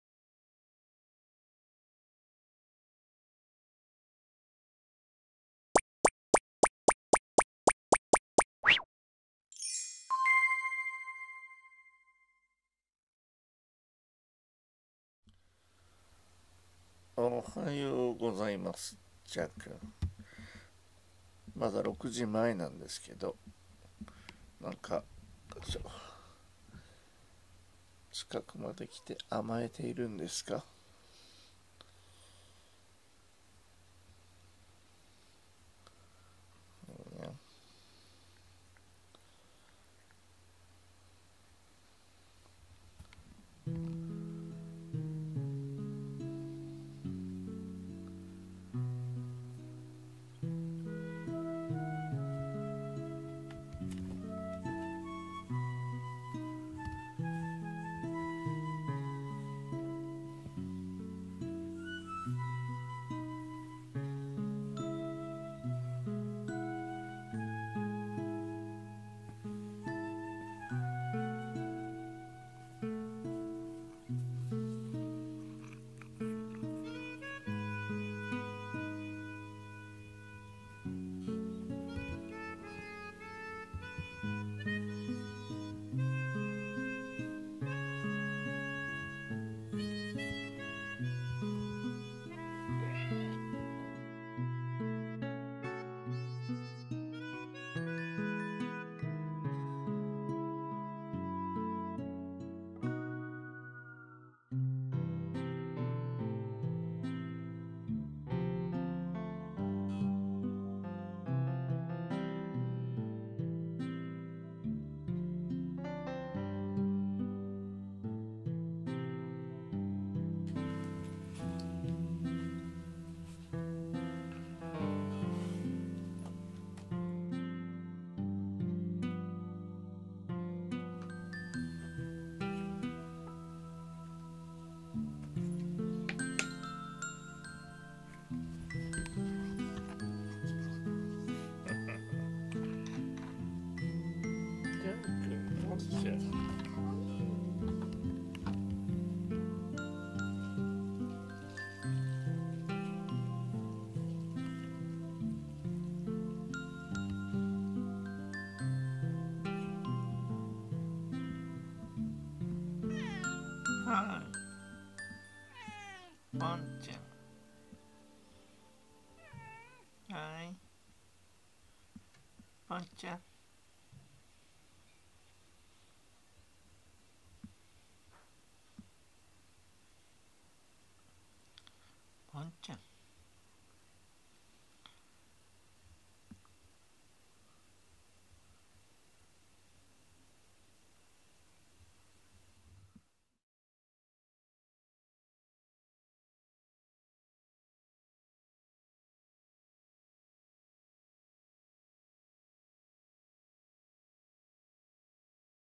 なぜ人は我が猫が世界一美しいと思うのでしょうか？【Jean & Pont 2481】2021/5/10 保護猫ジャンけんポン
親の欲目という言葉がありますが、飼い主の欲目？
ジャンくんとポンちゃんが世界一美しいと思っている夫婦なんです。
人の目は思い込みによってそれぞれ違う世界を見ているということでしょうか？
だったら世界は１つじゃないじゃないですか！